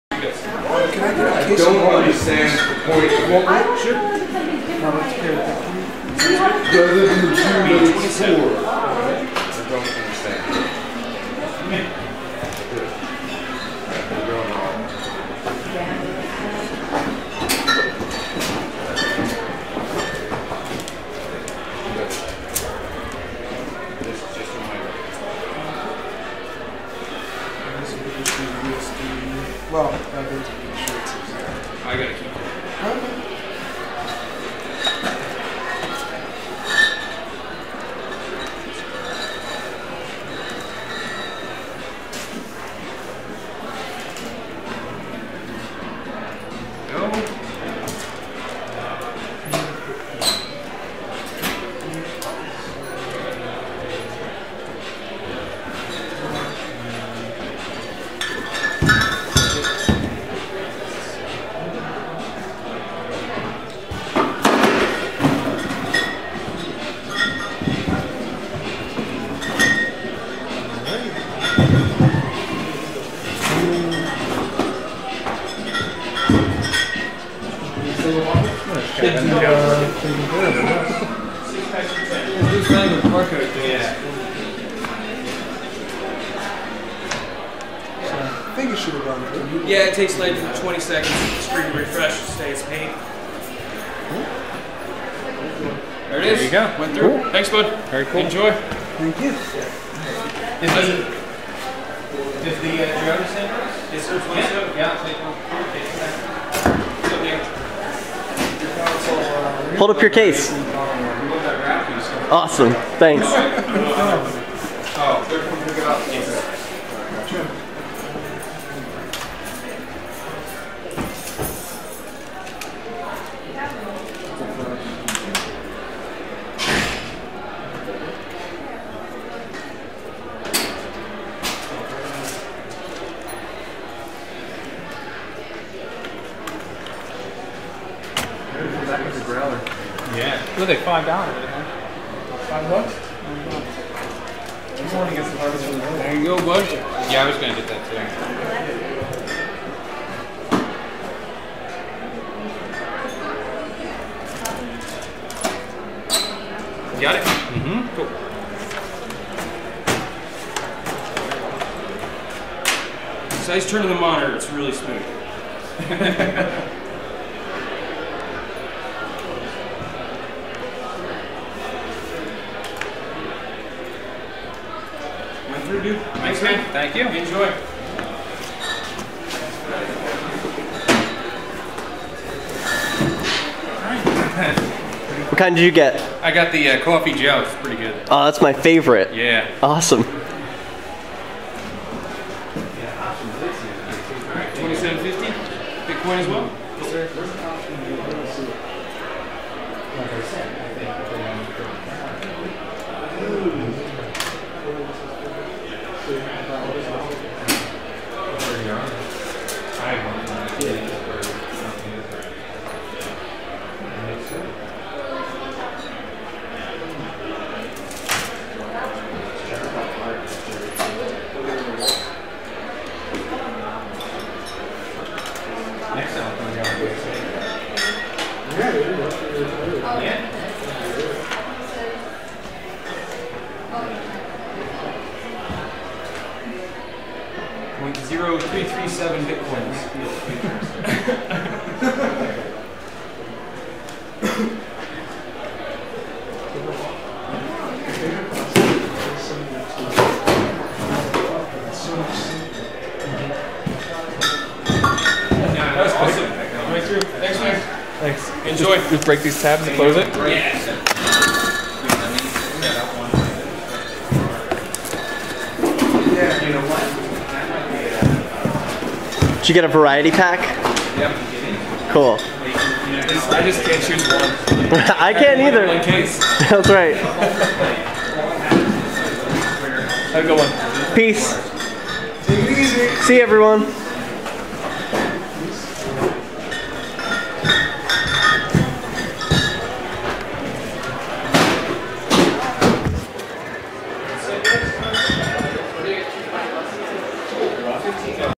Bitcoin Beer | Phila Brewing Co Tour | Part 18
February 1, 2014 Members of the Bitcoin Philadelphia group took a tour of the Philadelphia Brewing Company in Kensington, Philadelphia, the site of the first-ever brewery in the world to accept bitcoin directly.